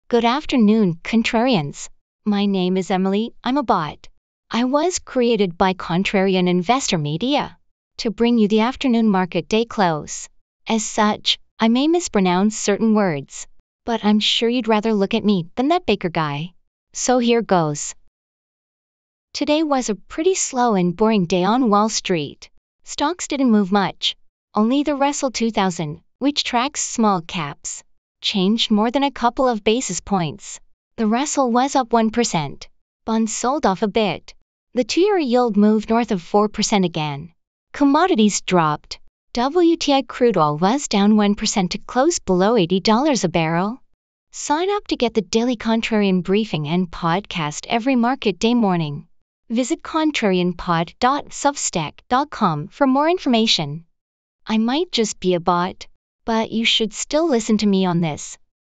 A Boring Day for Stocks: April 10 Wrap-Up With AI Host
Good afternoon contrarians! We've created a news anchor bot to bring you a quick summary of the day's stock market activity.

Check it out and sign up for the Daily Contrarian, which is out every market day morning and voiced by a real person: